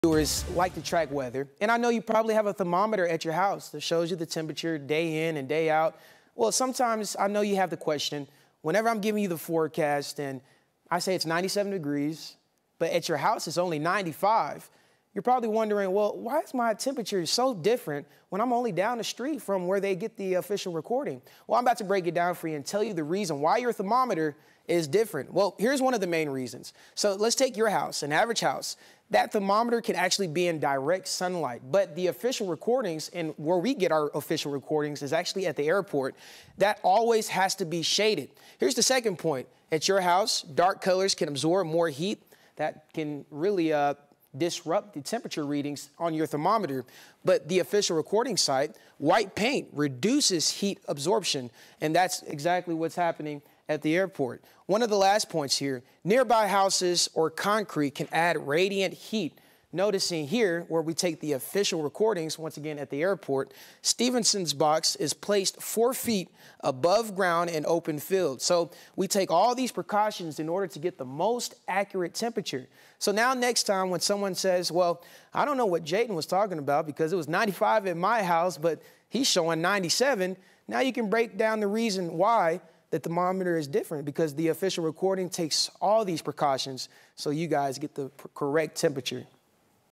What are the differences between thermometer readings?
9NEWS Meteorologist Jaden Knowles explains why your thermometer and the official thermometer readings are different.

9NEWS (KUSA) is located in Denver, Colorado.